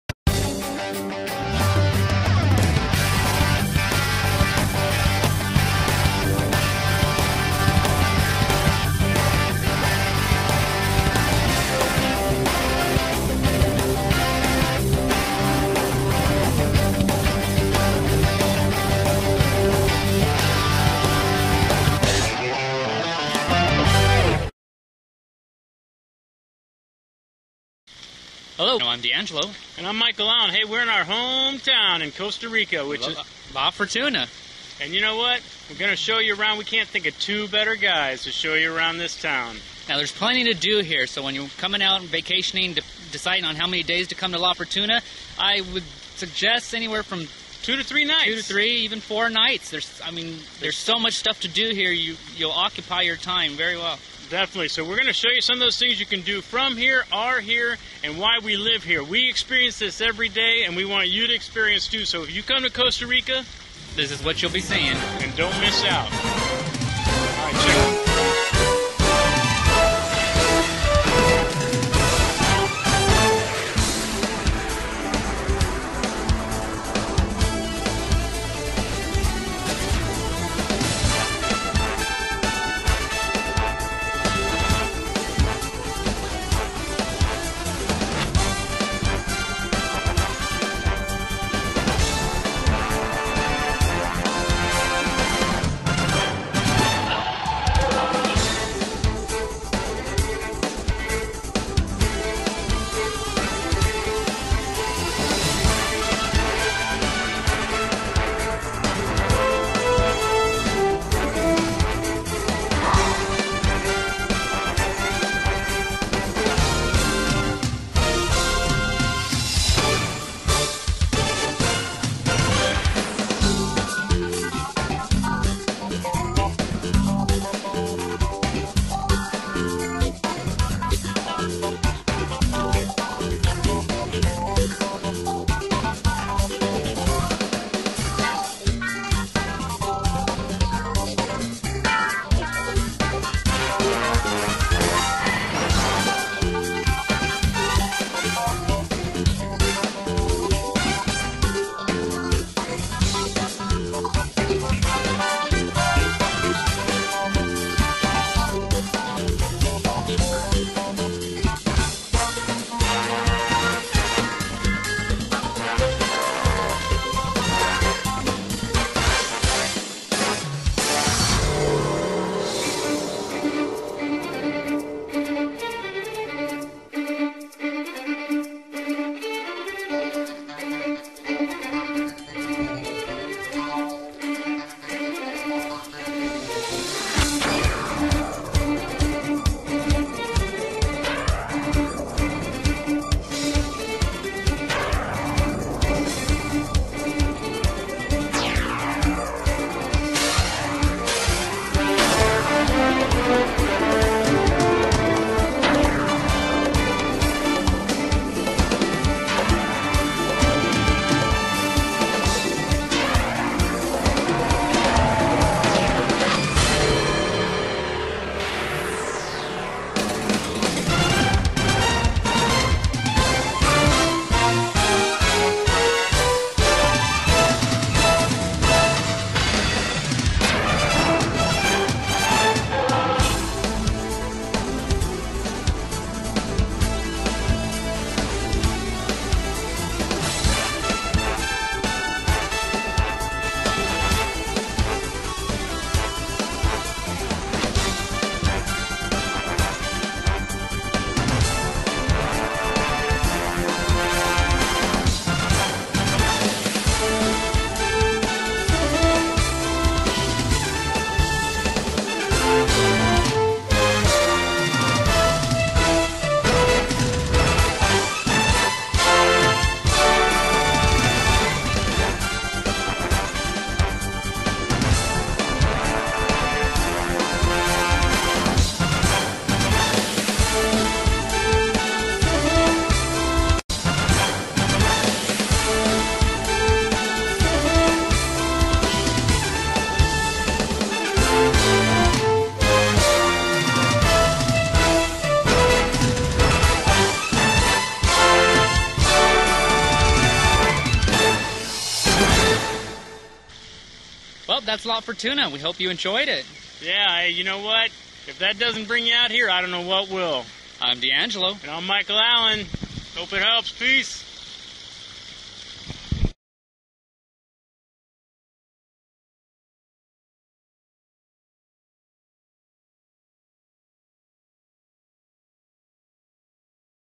La Fortuna Costa Rica
Hey everyone, DAngelo and michael alan from the Costa Rica Travel Channel. We live in La fortuna, right next to the Arenal Volcano. I can't think of two better guys to show you around. So check it out and see why we live here. The canopy tours, rafting, rappelling, hiking Cerro Chata and Fortuna Falls, biking, flora and fauna have nothing to do with it...hehe. If you're ever in the area, by all mean, stop in and bring the Cacique.

Lying at the eastern foot of the spectacular, Volcan Areanl, is the charming little tourist town (about 7,500) of La Fortuna. Recently this town has become quite the destination for volcano watchers from around the world. La Fortuna is the entry point to the road that connects the Arenal Volcano area to Lake Arenal and continuing on into Guanacaste. The magnificent Arenal Volcano is basically what put the little former farming town of La Fortuna on any visitors to Costa Ricas map, and prior to 1968, it was solely a agricultural town. Now it is one the premier tourist destinations in Costa Rica.

The history of La Fortuna and the Arenal Volcano have been connected since Volcan Arenal reared it massive head and awoke after a 400 year slumber and violently erupted in 1968 with the largest eruption in its recent history. It had been dormant so long it was thought by the locals to be a mere mountain. Actually, Arenal is an almost perfect cone, soaring 1,633 meters above sea level and has a crater 140 meters deep formed over an ancient caldera. The volcano dates back to about 3 million years ago. Arenal spewed ash, rocks and gas for 3 consecutive days and when the smoke finally cleared the villages of Tabacon, Pueblo Nuevo and San Luis were no more. From this catastrophic tragedy, La Fortuna (meaning the Fortune), was born as the town center and village for the Arenal Volcano area and like moths to a flame, tourists from around the world started descending on La Fortuna in search of fiery night skies.

La Fortuna has grown leaps and bounds in recent years all due to the lucrative tourism industry. Farmers sold their land and cabinas popped up out of nowhere and even without an active volcano spewing smoke, ash and lava overhead, La Fortuna would still be a great place to relax and kick back for a few days. Today, La Fortuna offers everything for the Costa Rica traveler and despite the influx of tourists the town still retains that cool vibe and real Tico fee. Accommodations from the 5 star Tabacon to a $10. per night hostel; and of course everything in between. There is easy access to rejuvenating hot springs, horseback riding to waterfall, white-water rafting, waterfall rappelling, hiking, volcano tours, canopy tours, bungee jumping, cave explorations, butterfly gardens and eco-tours, the list goes on and on. Its no wonder La Fortuna has become one of the top tourist destinations in Costa Rica.